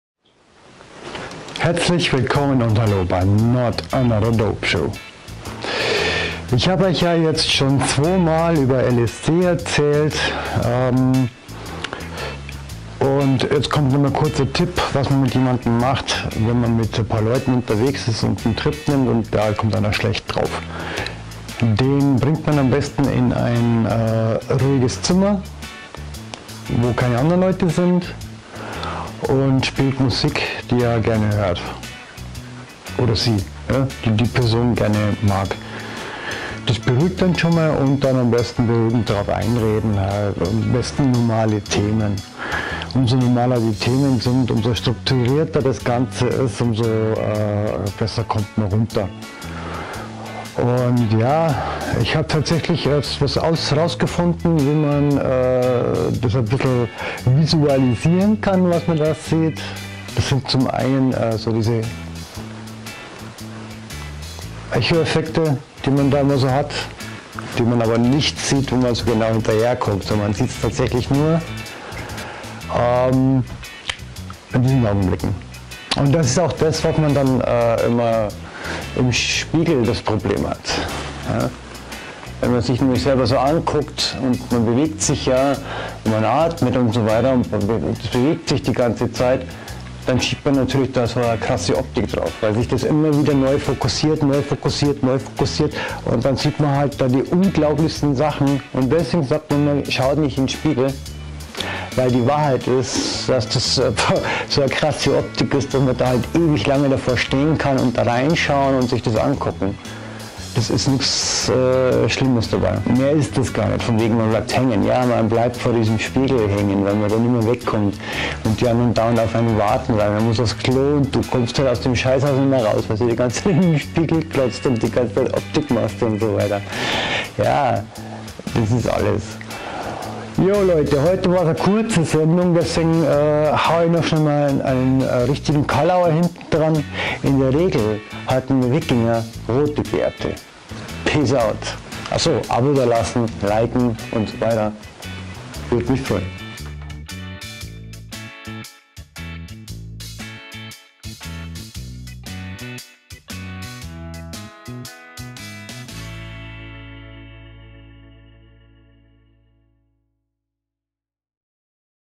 LSD Optik im VIDEO
LSD, bicycle-day, runterkommen Servus und Hallo bei Not another Dope Show,

Heute geht es um heftige LSD Erfahrungen, bzw was man machen kann wenn jemand "schräg" drauf kommt UND ich zeige LSD Optik im VIDEO.

Hater Kommentare werden kommentarlos gelöscht. Kritik lasse ich stehen.